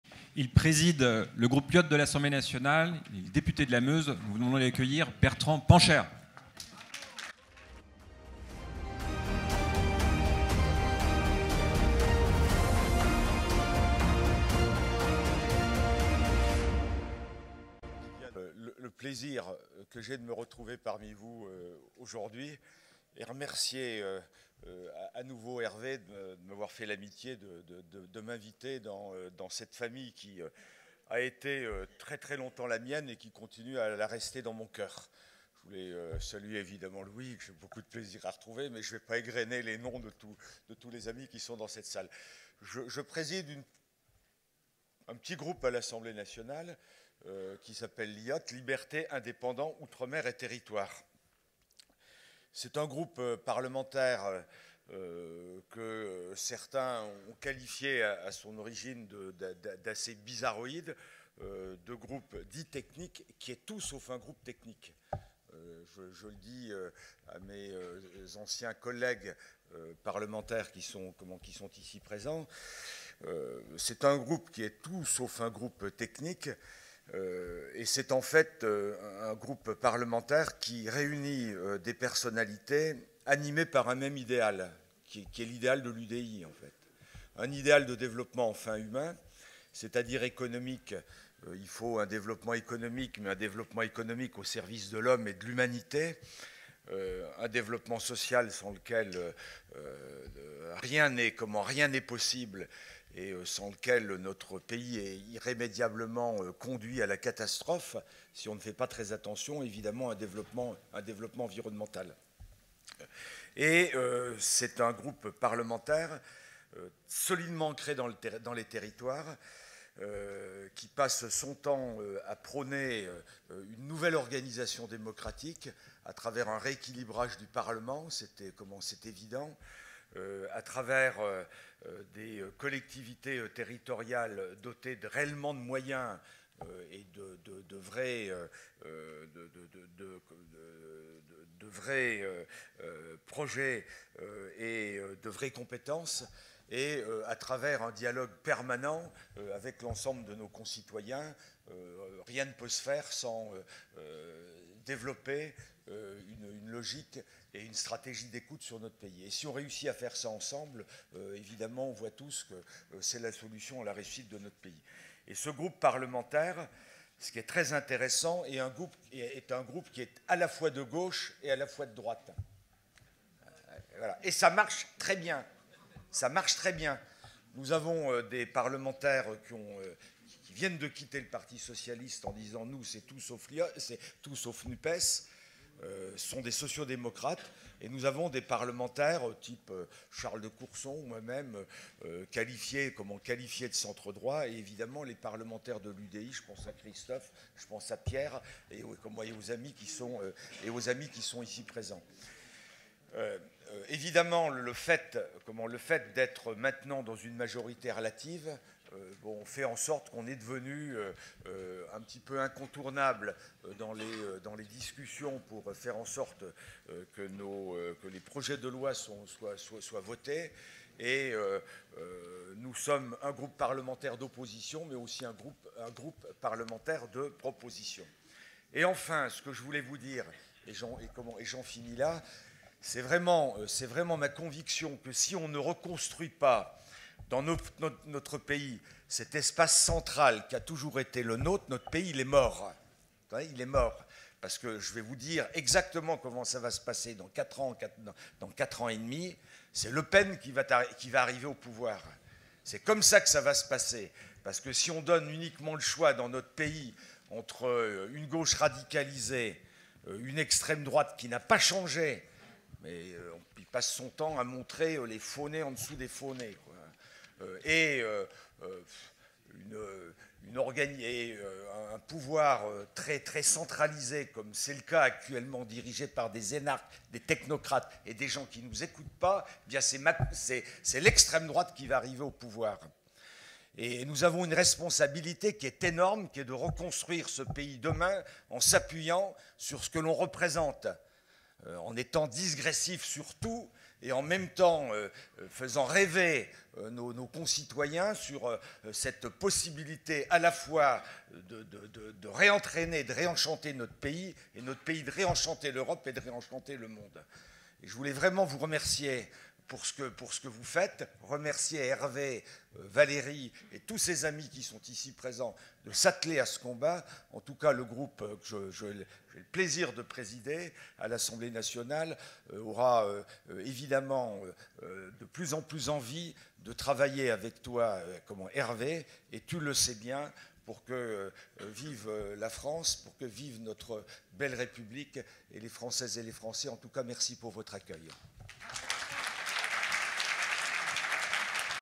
Congrès UDI du 10 déc. 22 : Discours de Bertrand Pancher, Président du groupe LIOT à l'Assemblée
Retrouvez l'intervention de Bertrand Pancher, Président du groupe Libertés, Indépendants, Outre-mer et Territoires de l’Assemblée nationale et Député de la Meuse au Congrès de l'UDI du 10 décembre 2022